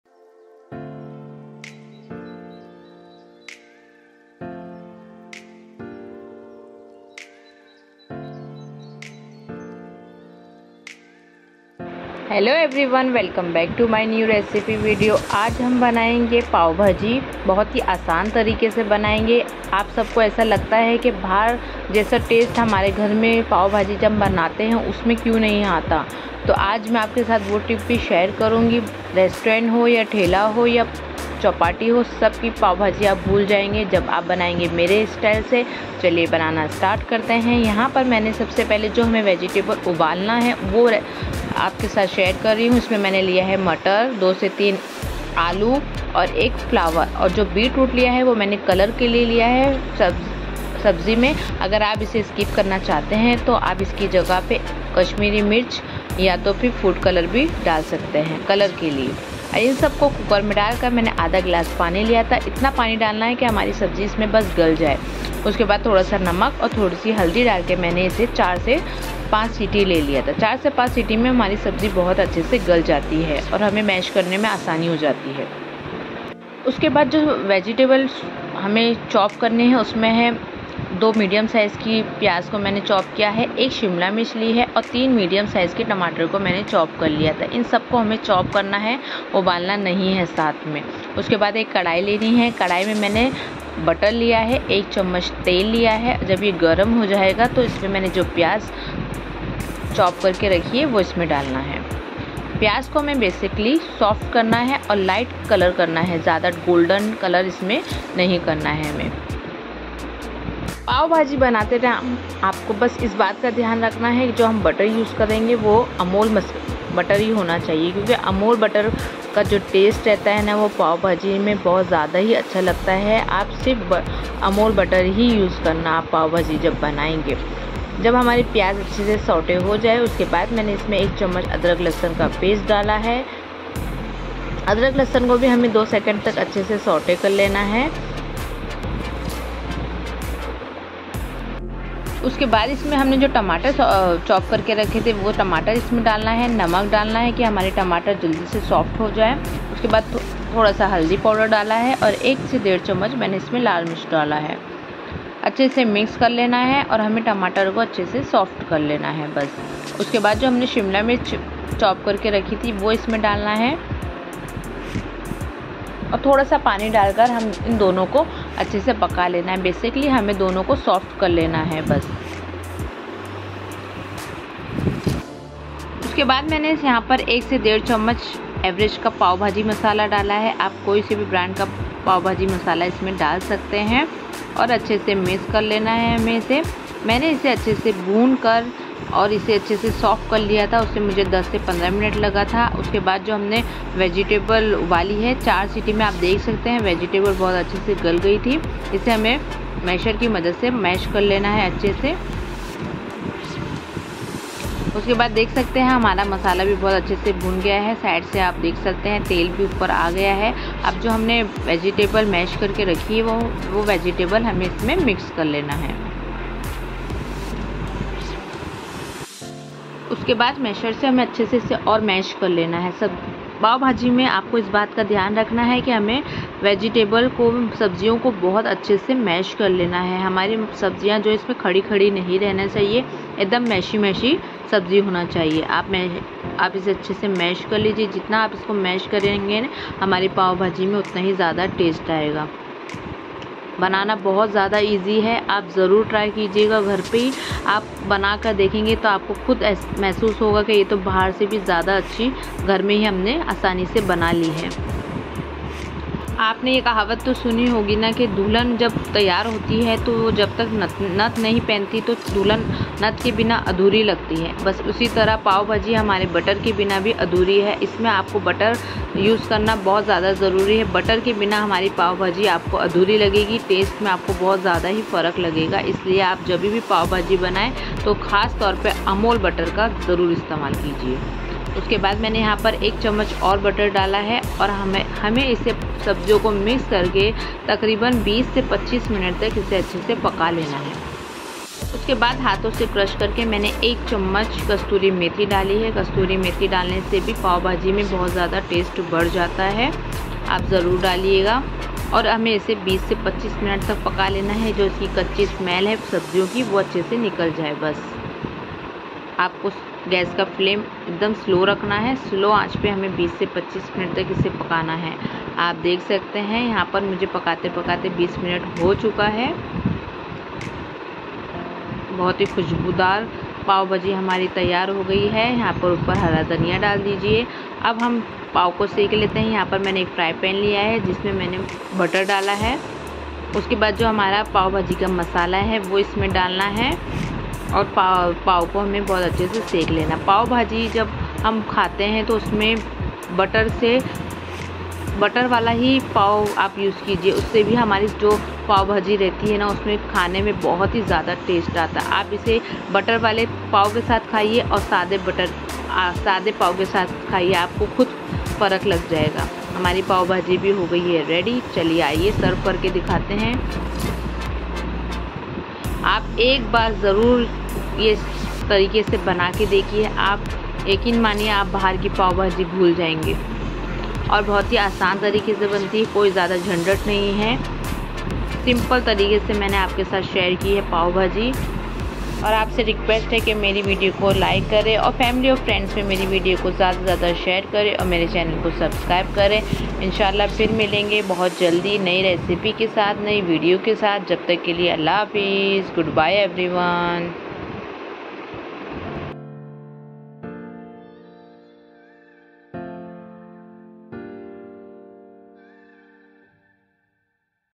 Mumbai Famous Pav Bhaji ab Ghar pr banaye mere style se#simplykhanakhazana
hello everyone

aaj aapke liye lekar aayi hu pav bhaji recipe ek baar banaoge toh bahar ki pav bhaji bhul jaoge isliye zarur try kare aur video ko like share and channel ko subscribe kare

plz follow my Instragram Id